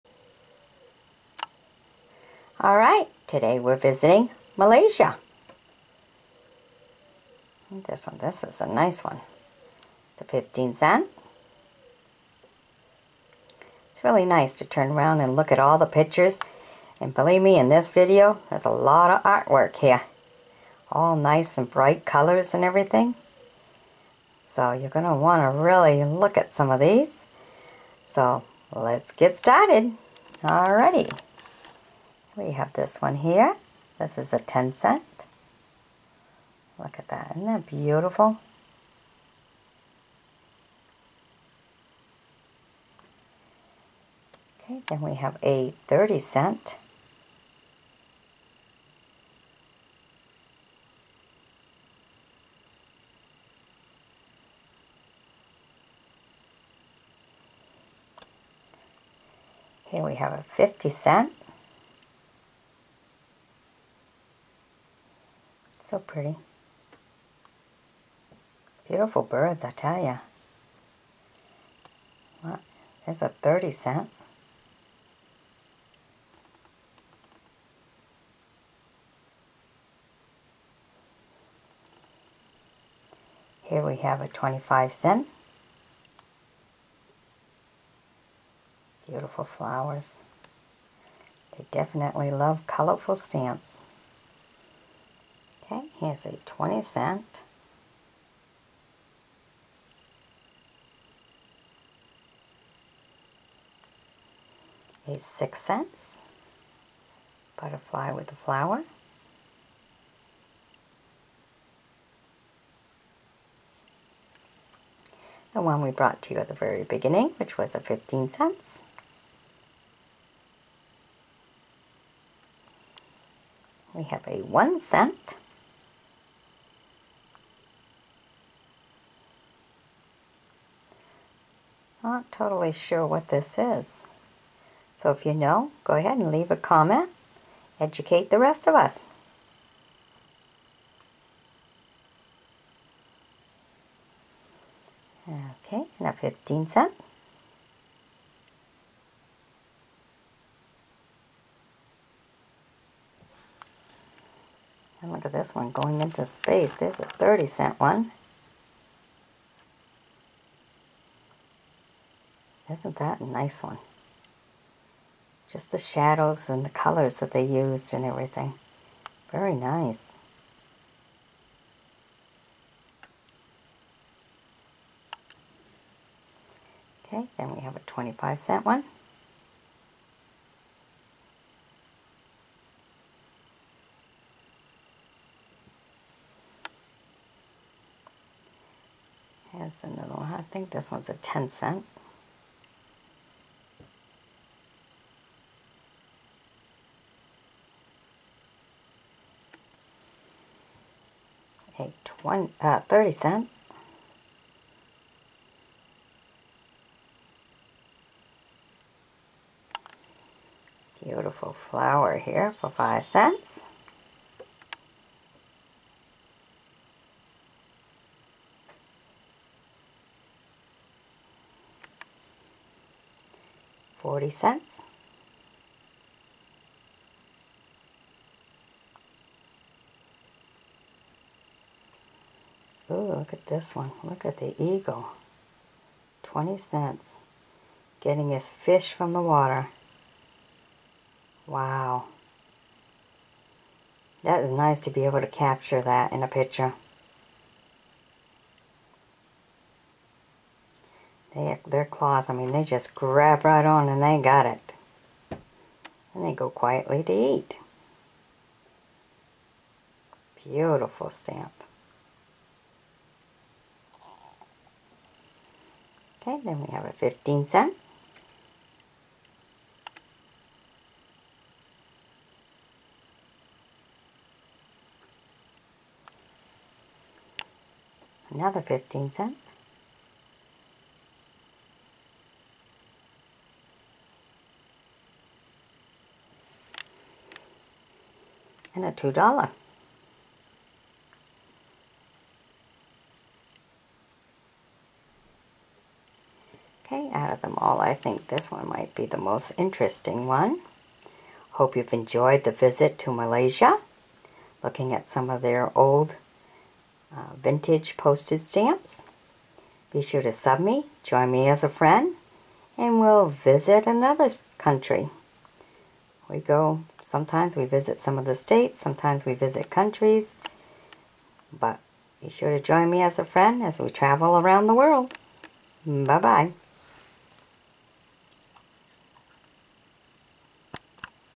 My Colorful Old Malaysia Postage Stamps
A great collection of some very colorful Malaysia old postage stamps.

To view over 3,000+ of my old and rare postage stamps with videos attach From around the world please